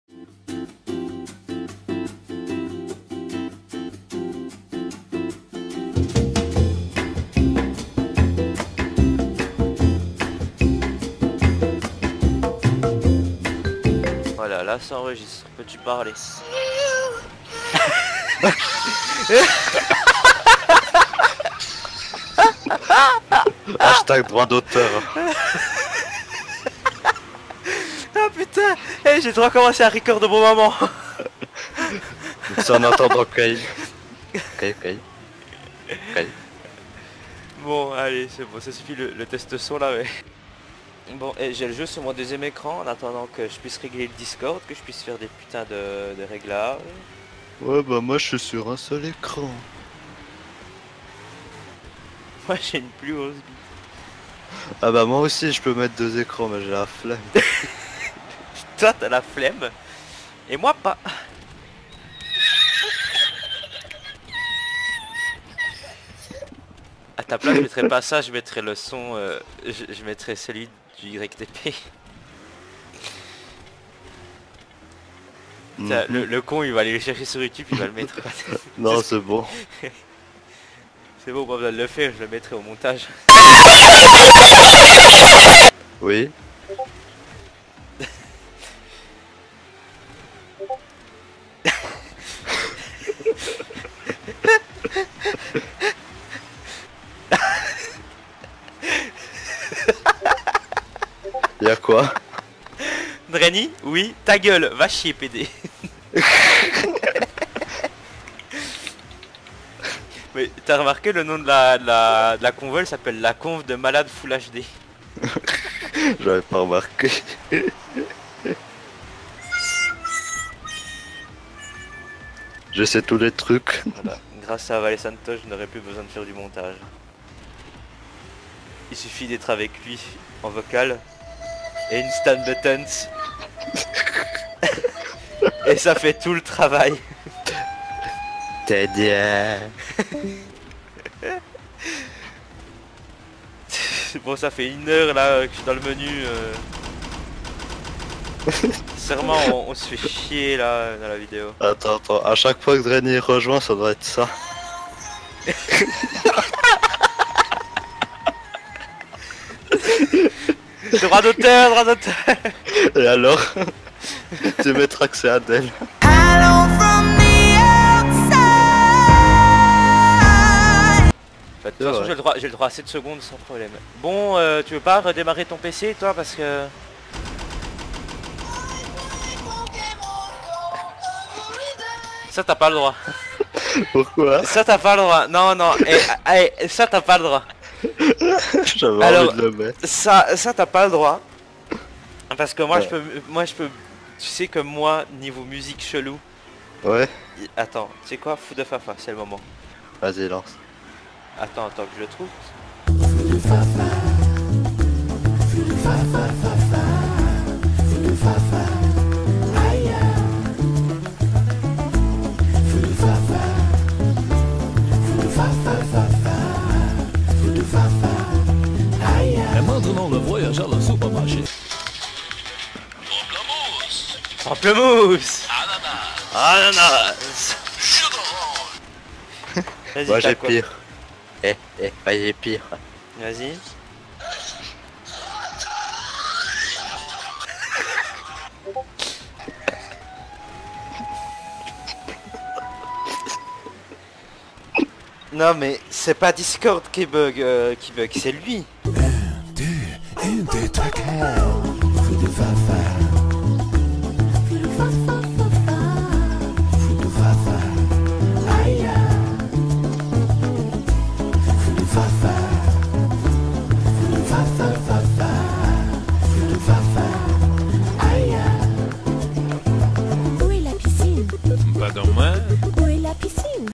Les Problèmes de KayX avec Discord
Voilà ce qui arrive quand on s'ennuie en attendant KayX avec Vakesantos...

Montage: Laolyth

Miniature: Laolyth.

Capture: Laolyth.